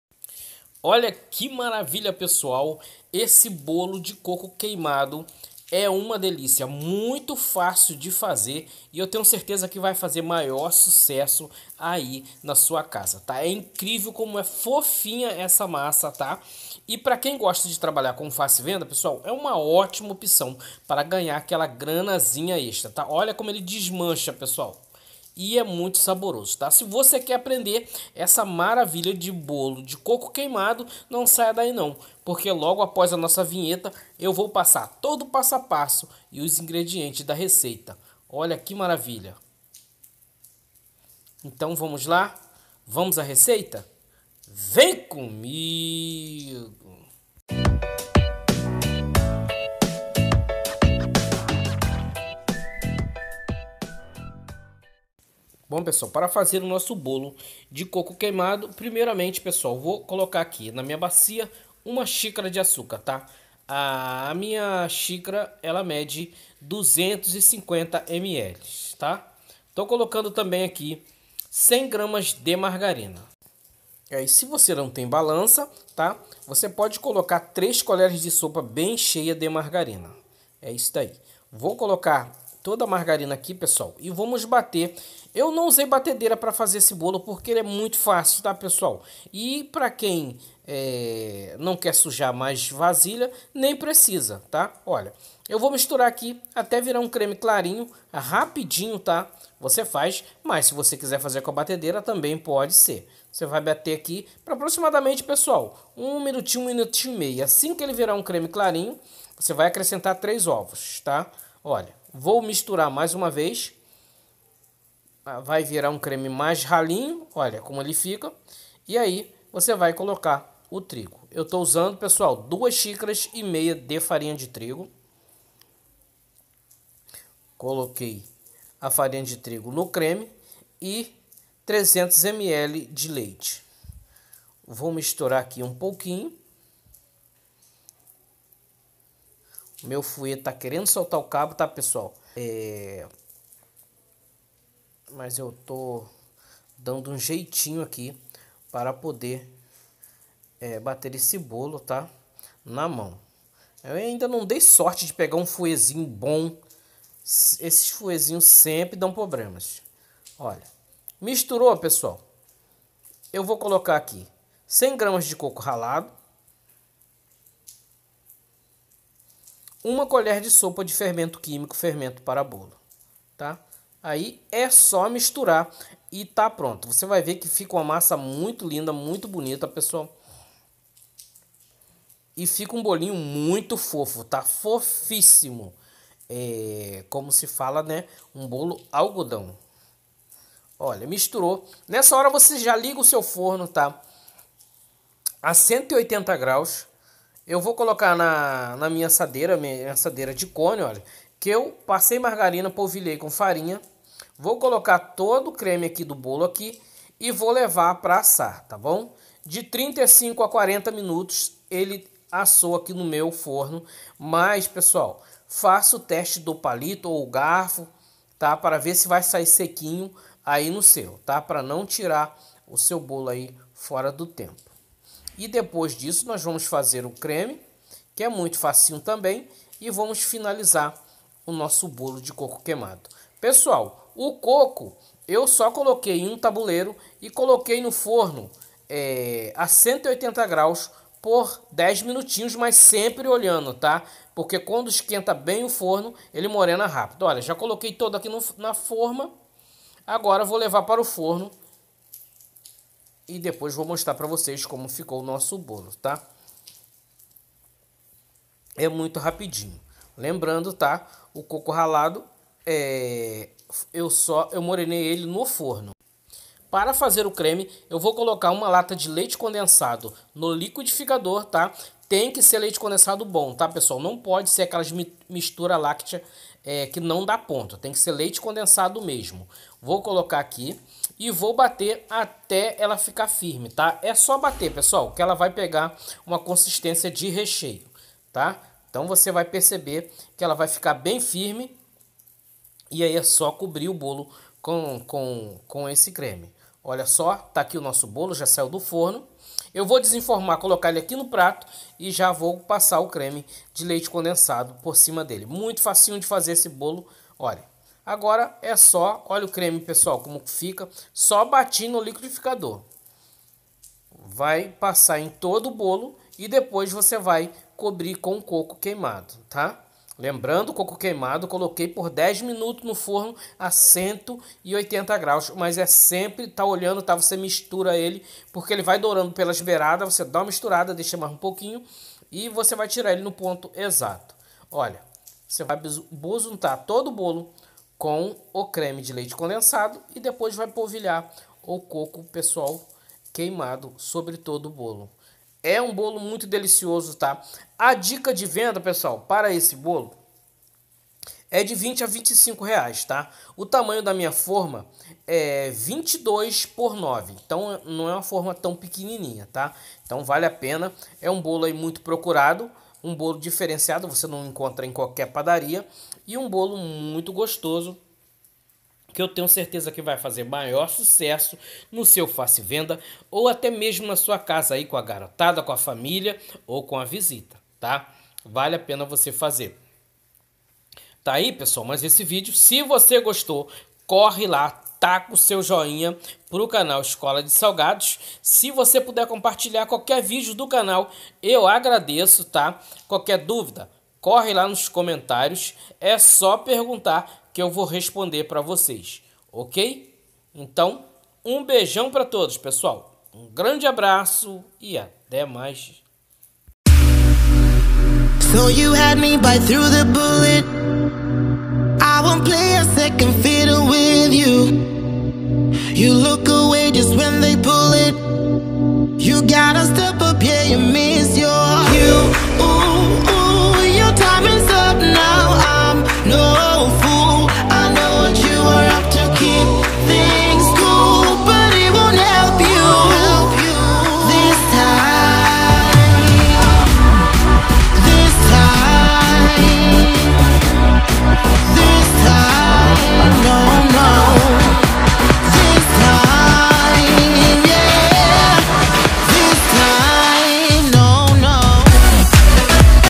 Como fazer bolo de coco queimado fácil e delicioso
Ingredientes
2 xicaras e meio de trigo
1 xicara de açúcar
300 ml  xicara de leite
3 ovos
100 Gr de margarina
1 colher de fermento químico (  para bolo )
100 Gr de coco ralado

Para finalizar
1 caixinha de leite condensado
200 Gr de coco queimado

PESSOAL SE POSSÍVEL ME AJUDEM NO MEU OUTRO CANAL ONDE IREI FAZER VÁRIOS TIPOS DE COMIDAS